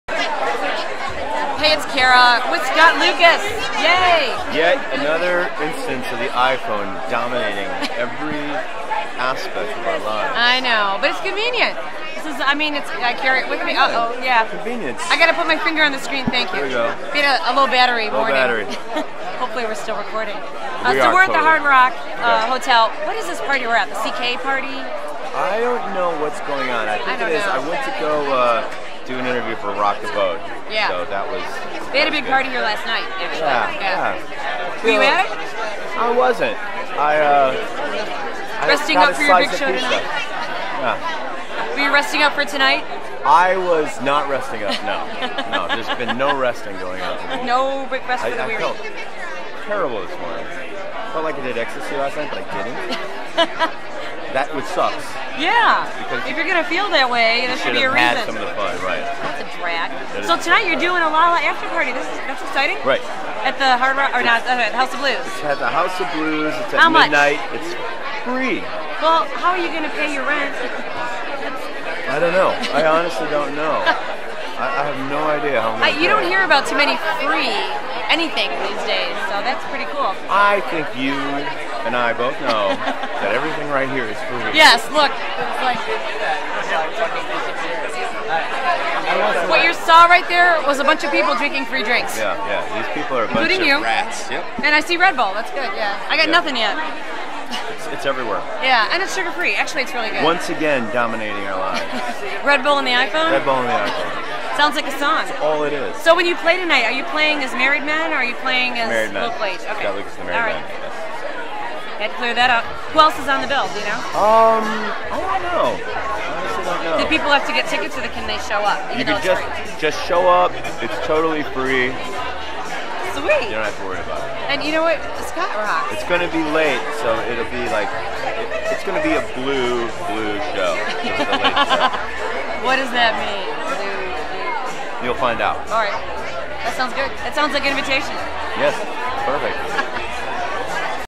Scott Lucas hangs at Lollapalooza
Cara Carriveau chats with Scott Lucas (Married Men, Local H) at the CK Lounge at the Hard Rock Hotel during Lollapalooza.  He spills about the FREE show he is playing tonight at the House Of Blues! (Backporch Stage)